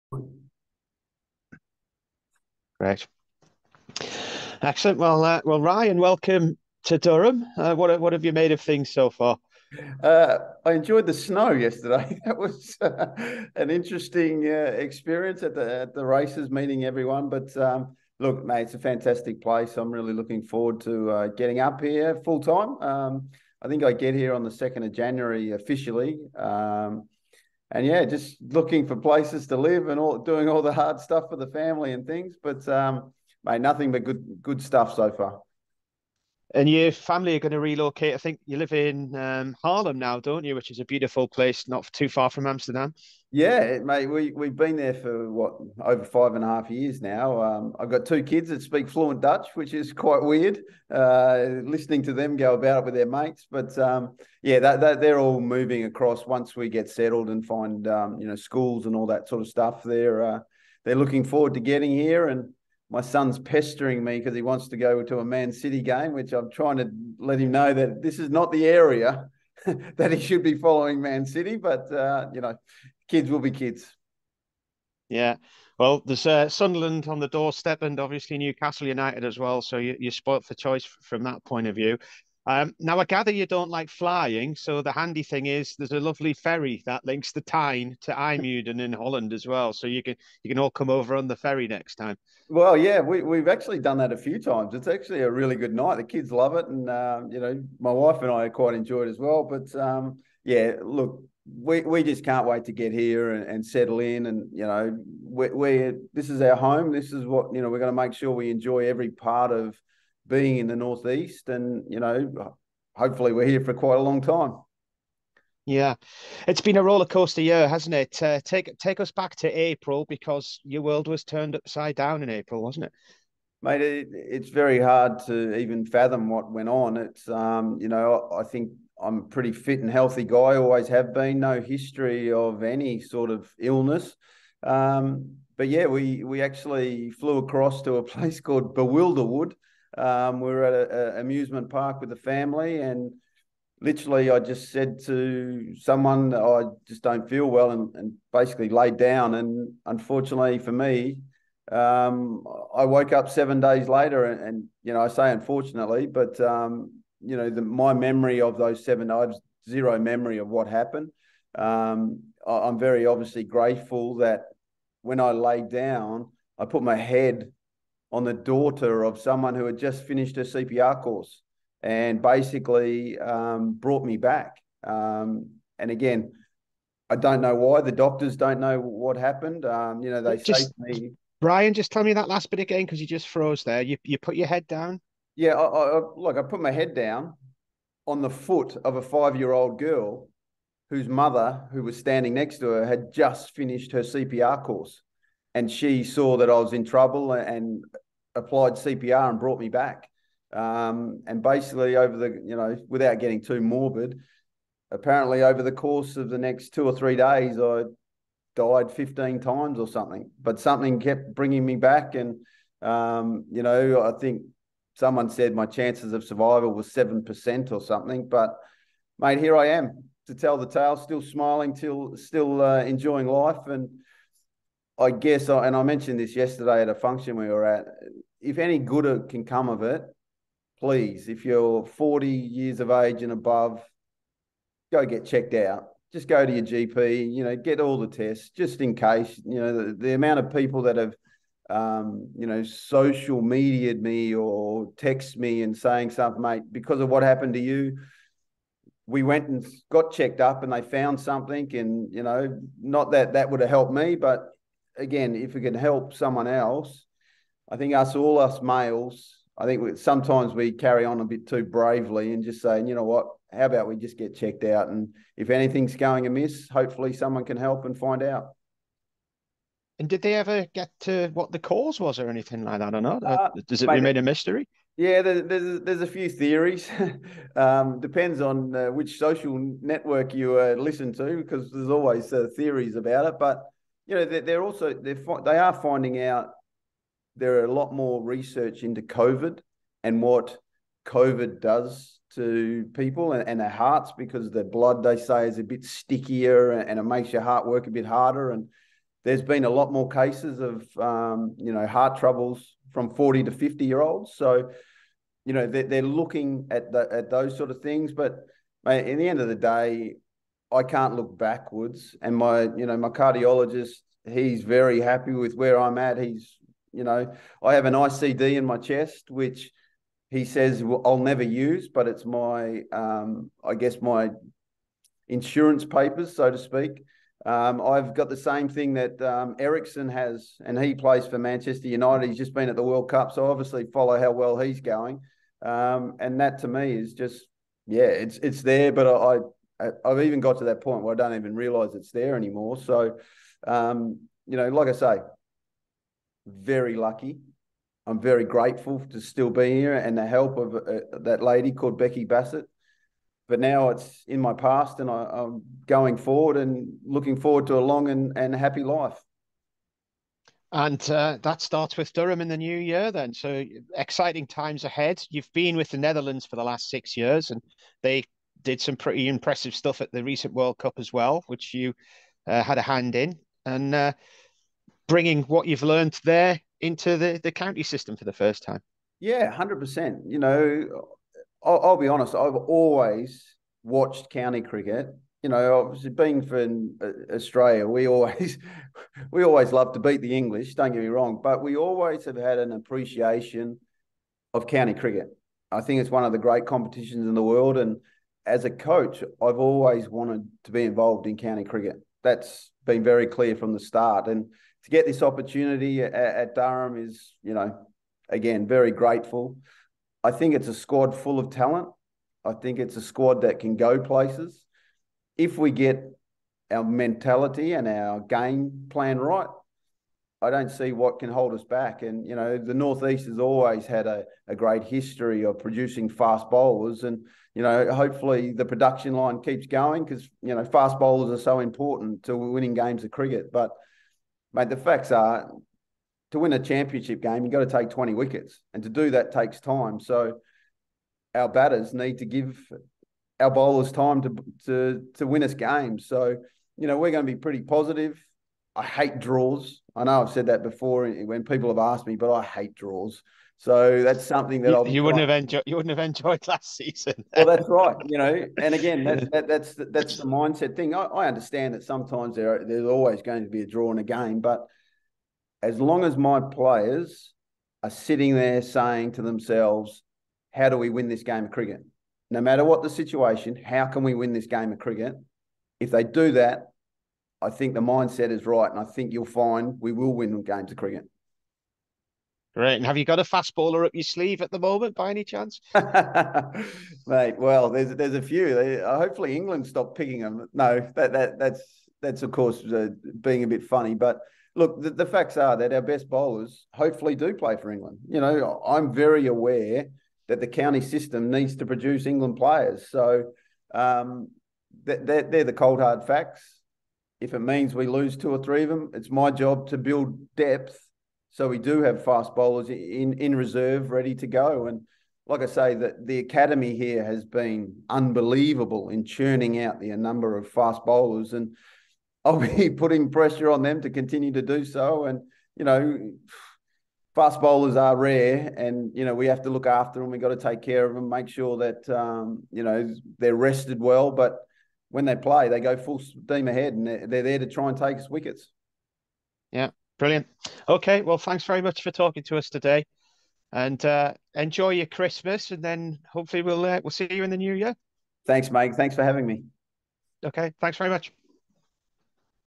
Ryan Campbell catches up with BBC Newcastle on his arrival at Durham Cricket
New Durham Head Coach caught up with BBC Newcastle commentator Martin Emmerson following Ryan Campbell's appointment.